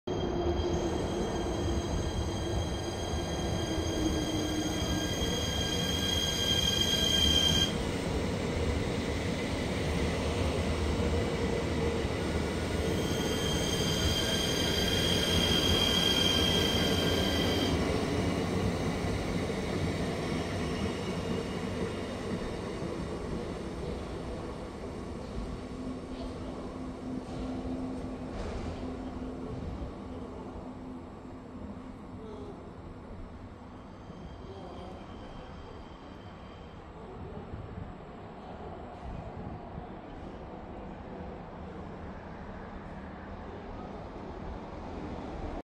[철도풍경] 코레일 서해선 ITX-마음 당역종착 열차 서화성역 출발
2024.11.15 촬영

서화성행 ITX 마음 열찬데 역에서 내리는 사람은 별로 없습니다.